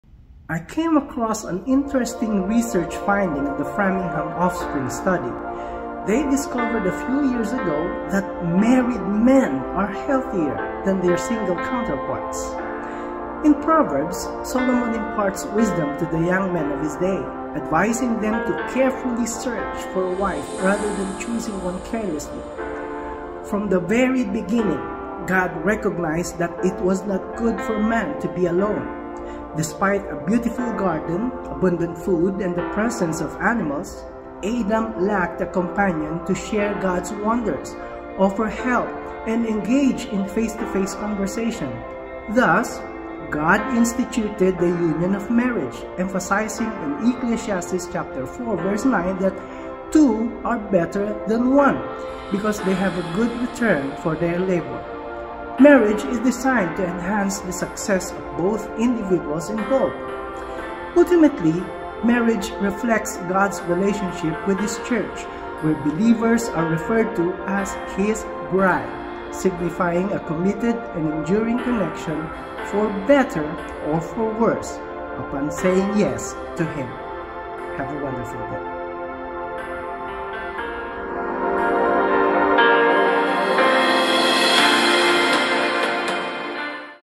FINDING A WIFE
Marriage, advised in the Bible and supported by studies, fosters health and companionship, mirroring the enduring commitment between believers and their Creator.